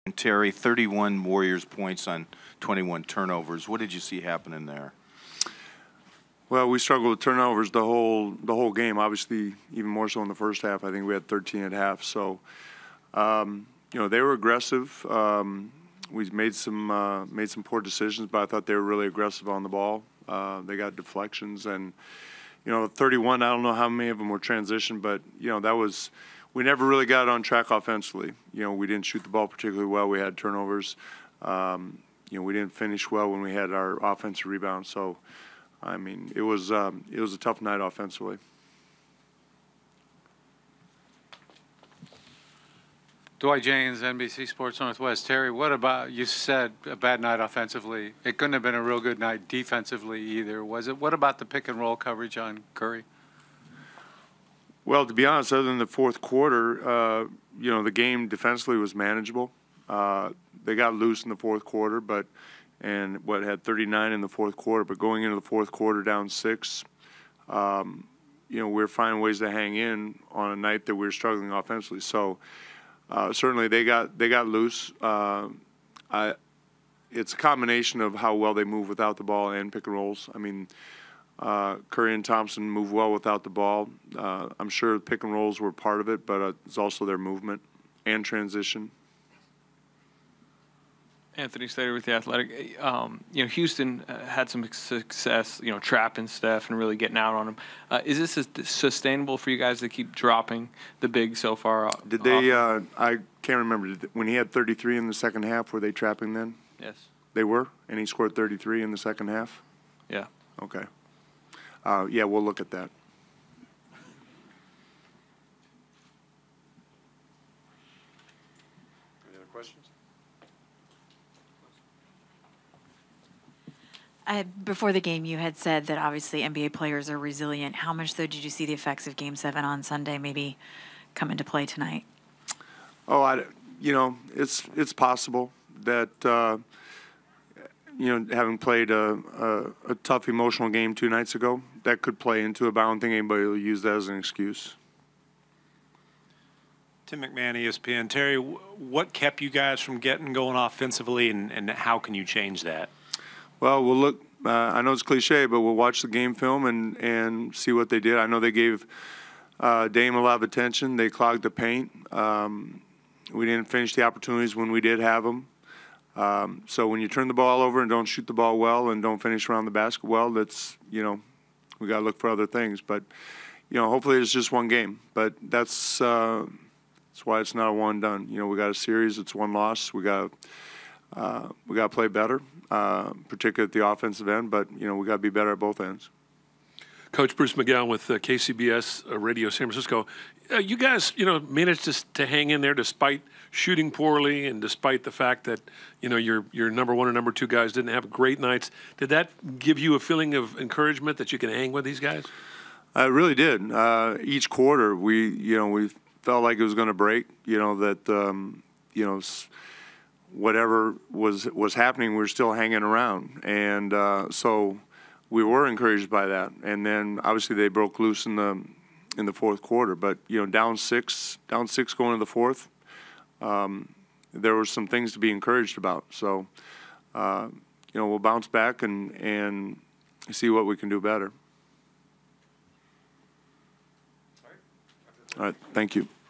Terry Stotts Postgame Interview - Game 1 | Trail Blazers vs Warriors | 2019 NBA Playoffs
Portland Trail Blazers vs Golden State Warriors - Game 1 | Western Conference Finals WCF | May 14, 2019 NBA Playoffs